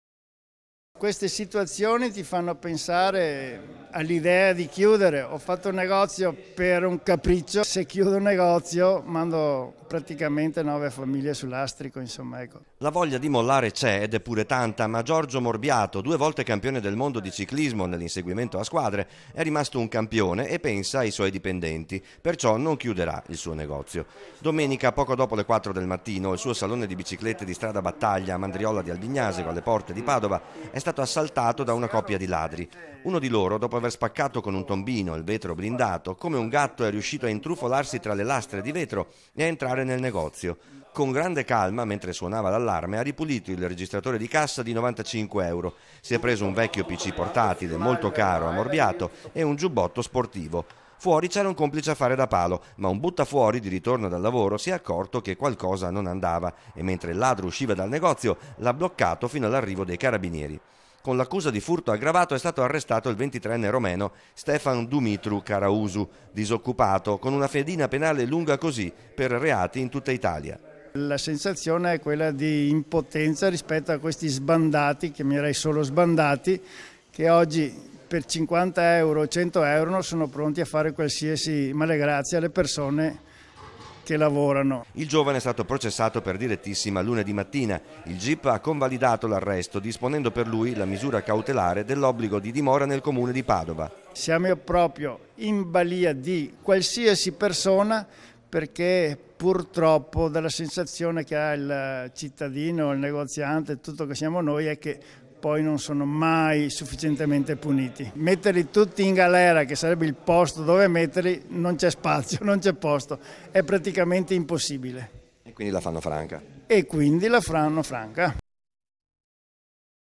TG (22/10/2018) - SPACCATA DA MORBIATO: «CHIUDERE? NO, AI DIPENDENTI CI TENGO»
TG (lunedì 22 ottobre 2018) - La voglia di mollare c'è ed è pure tanta ma Giorgio Morbiato, due volte campione del mondo di ciclismo nell'inseguimento a squadre, è rimasto un campione e pensa ai suoi dipendenti perciò non chiuderà il suo negozio. Domenica poco dopo le quattro del mattino il suo salone di biciclette di strada Battaglia a Mandriola di Albignasego, alle porte di Padova, è stato assaltato da una coppia di ladri. Uno di loro, dopo aver spaccato con un tombino il vetro blindato, come un gatto è riuscito ad intrufolarsi tra le lastre di vetro e ad entrare nel negozio.  Con grande calma, mentre suonava l'allarme, ha ripulito il registratore di cassa di 95 euro, si è preso un vecchio pc portatile molto caro a Morbiato e un giubbotto sportivo. Fuori c'era un complice a fare da palo. Ma un buttafuori di ritorno dal lavoro si è accorto che qualcosa non andava e mentre il ladro usciva dal negozio l'ha bloccato fino all'arrivo dei carabinieri. Con l'accusa di furto aggravato è stato arrestato il 23 enne romeno Stefan Dumitru CARAUSU, disoccupato con una fedina penale lunga così per reati in tutta italia.Il giovane è stato processato per direttissima lunedì mattina. Il GIP ha convalidato l’arresto disponendo per lui la misura cautelare dell’obbligo di dimora nel Comune di Padova.